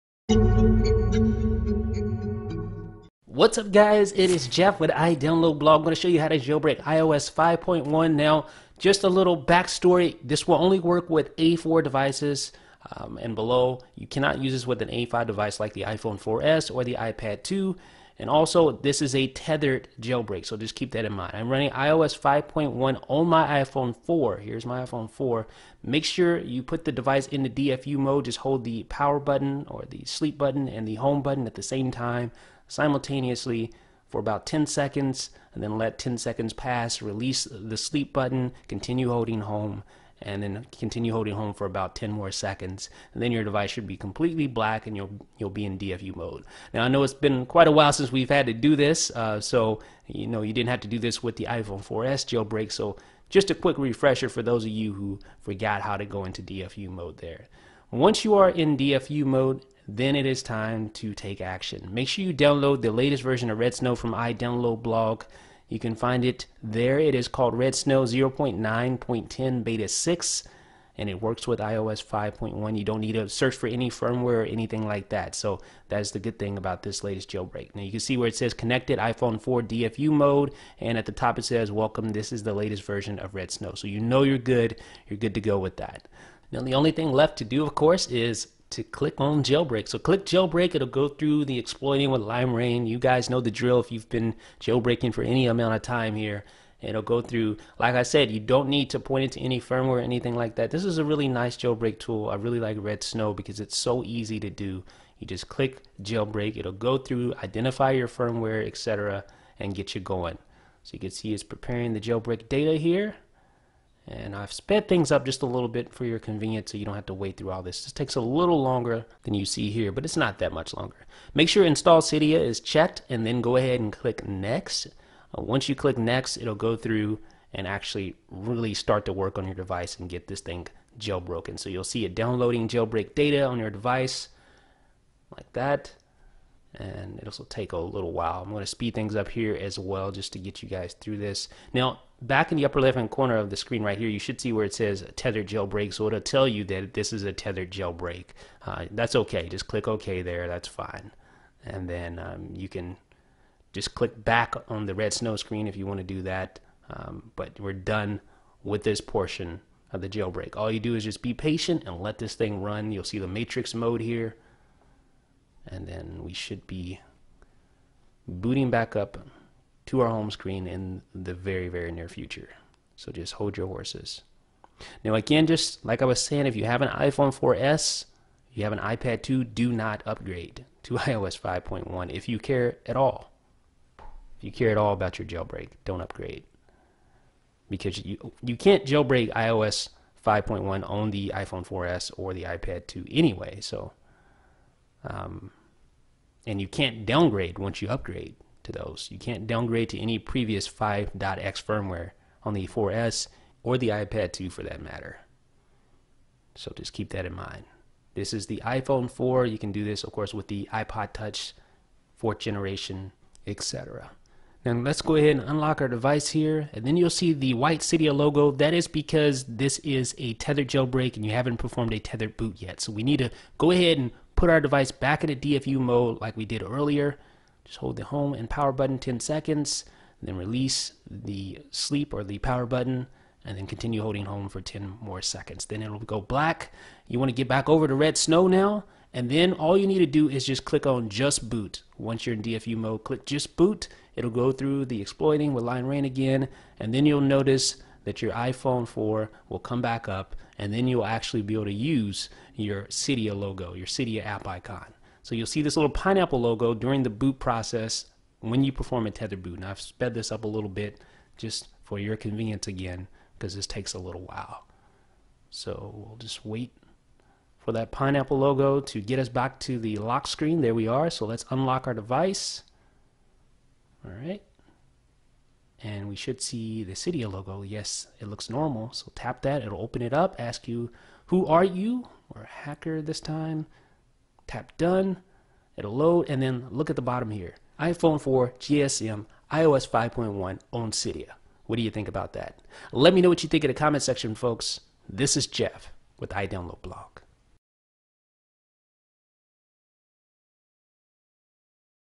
How to Jailbreak iPhone 4 iPhone 3GS iPod touch iPad 1 - Tutorial
Tutorial on how to jail break your iphone, ipod touch, or ipad 1.